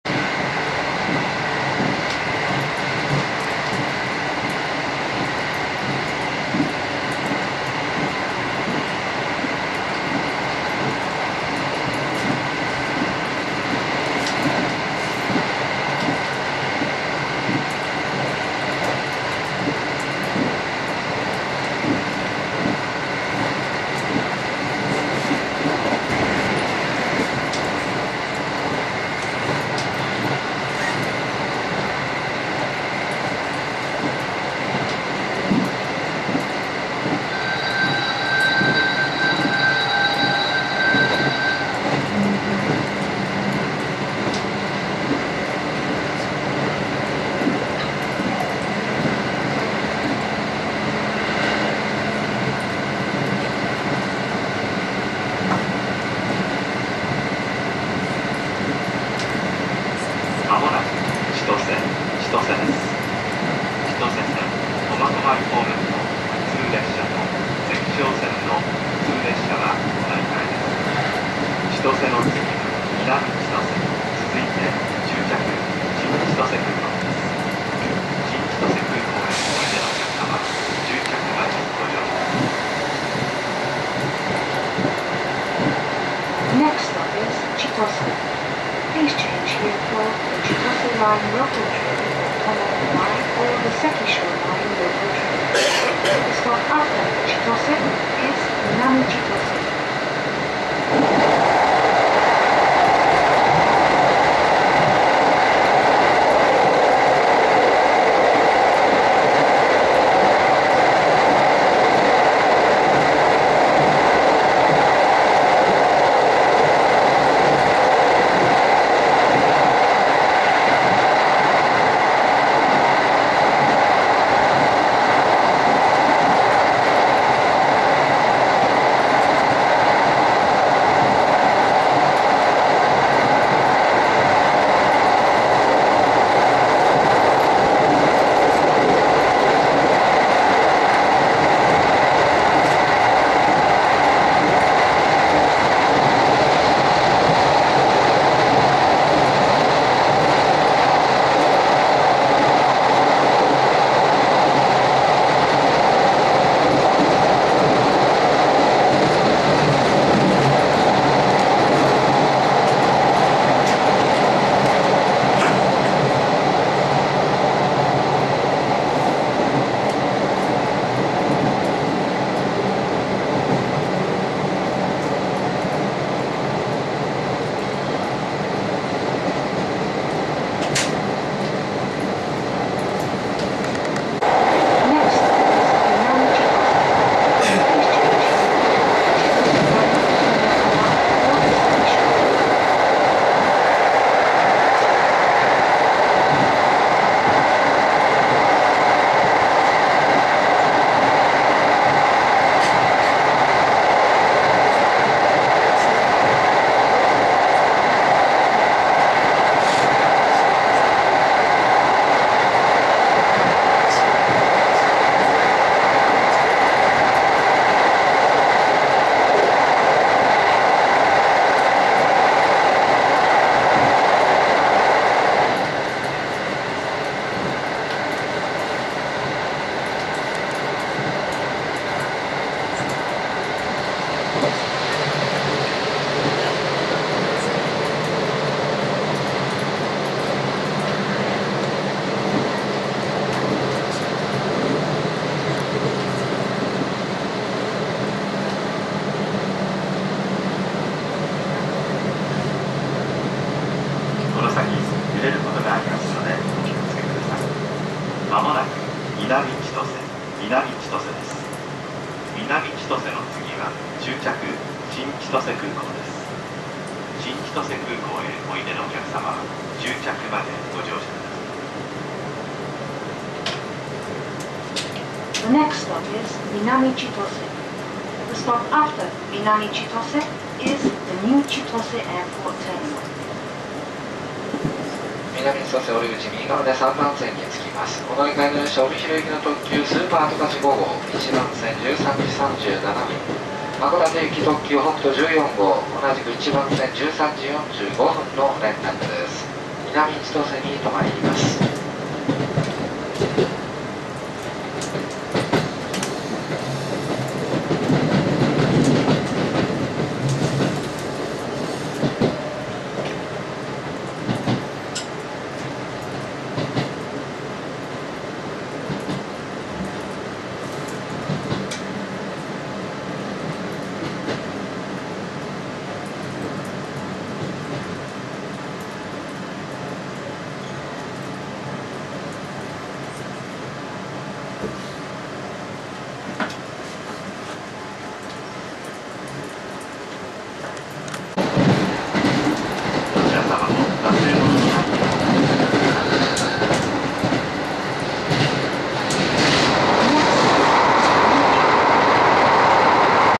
札幌新千歳空港間の車窓
ＪＲ線・新千歳線の札幌と新千歳空港駅間の車窓風景です。寝ており途中で気づきあわててカメラを回してます。住宅地の木々も色づき結構いい景色ですね。
Sony HDR-CR560Vで撮影、EDIUS6でM2TSへ変換。レートは20M程で高めです。
クリップをただつなげたでけで一切編集していません。音量も大きすぎかもしれません。